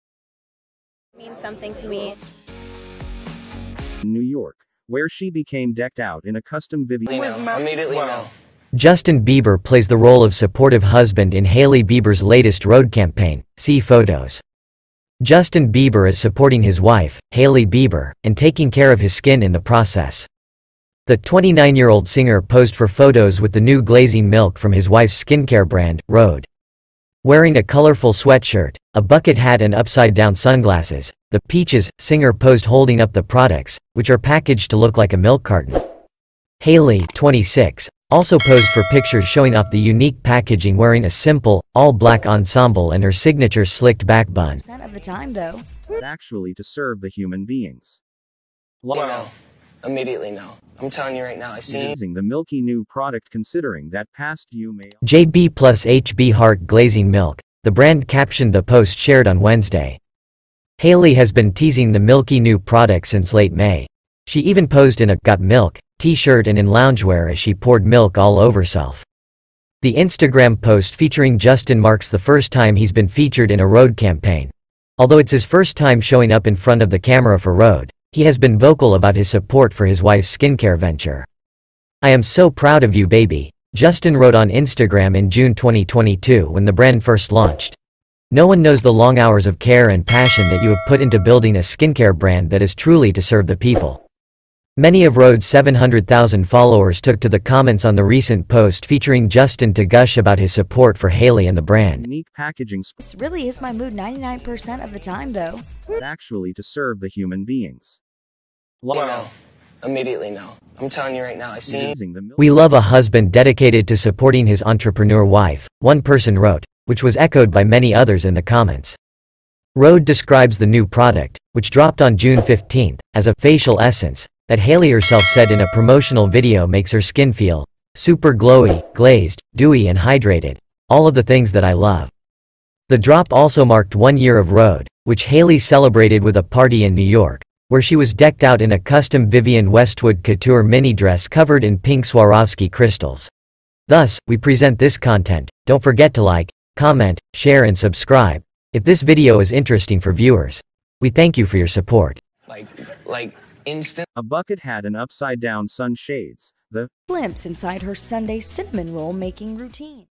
Justin Bieber Plays The Role Of Supportive Husband In Hailey Bieber Latest Rhode#news
Justin Bieber Plays The Role Of Supportive Husband In Hailey Bieber Latest Rhode
justin bieber,hailey bieber,celebrity news